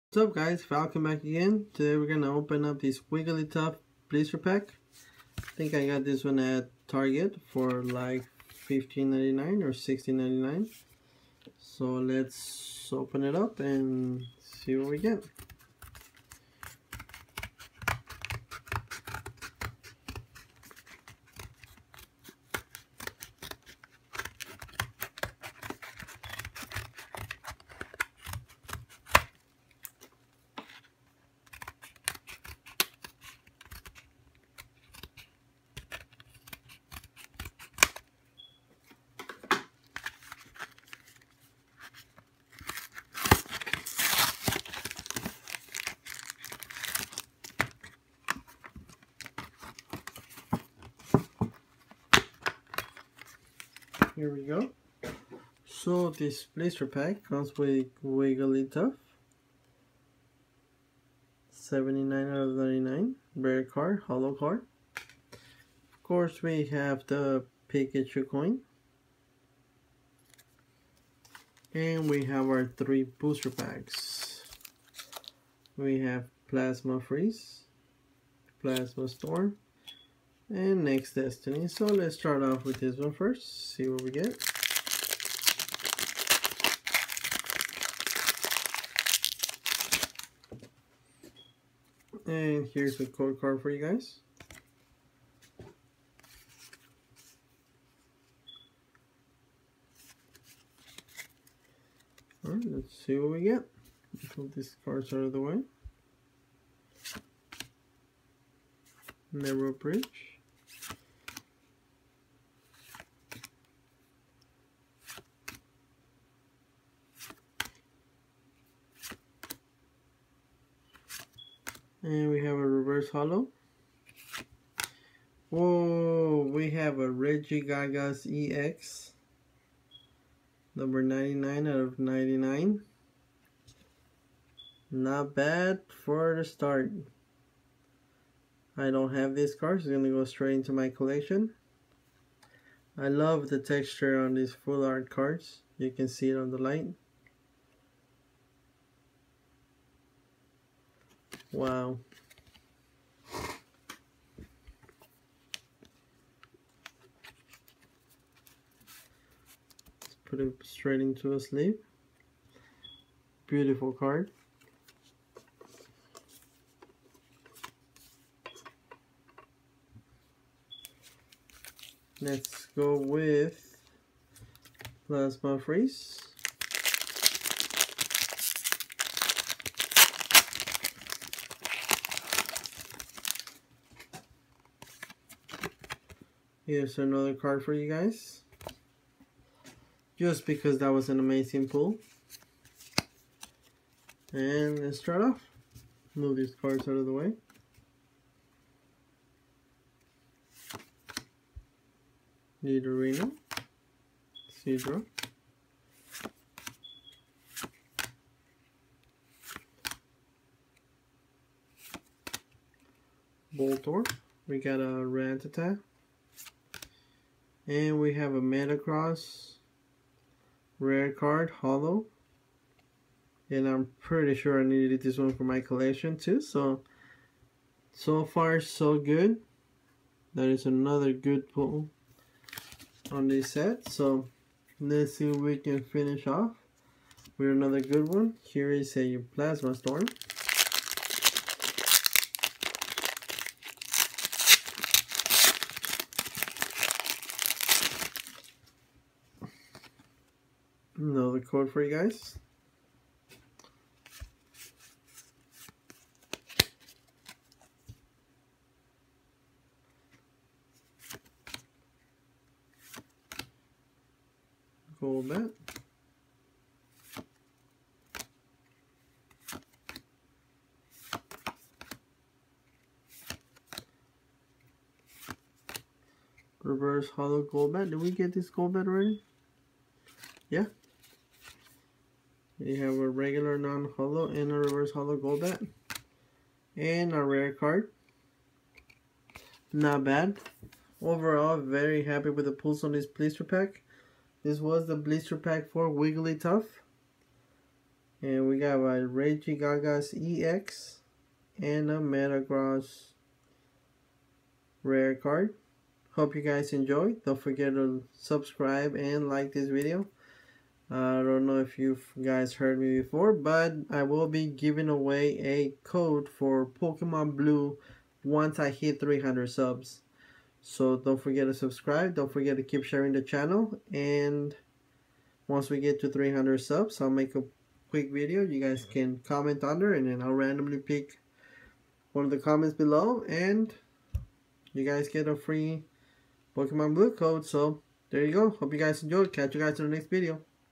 Pokemon TCG Black and White Wigglytuff Blister Pack!! ( FULL ART )
Opening a Black & white Wigglytuff blister pack with a plasma Blast and a Plasma Freeze, Next destinies packs, also has a Wigglytuff card and a Pikachu coin.

Once I hit 300 subs I will give out a pokemon Blue for nintendo DS code. keep sharing the channel guys.

Intagram.